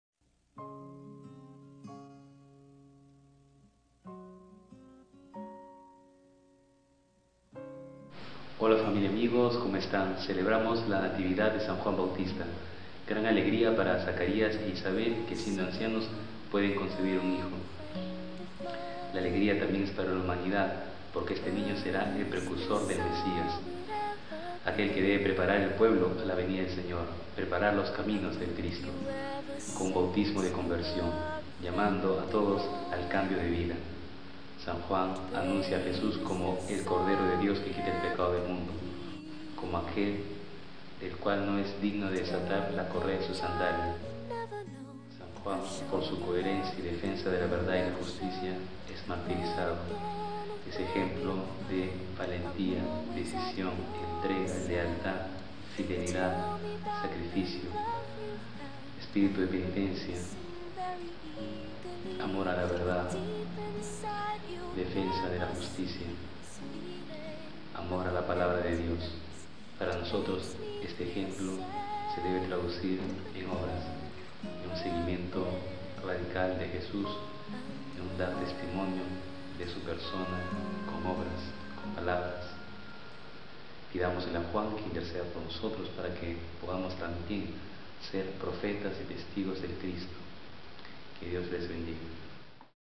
Natividad del Bautista (P. César Villar)
Reflexión sobre la figura de San Juan Bautista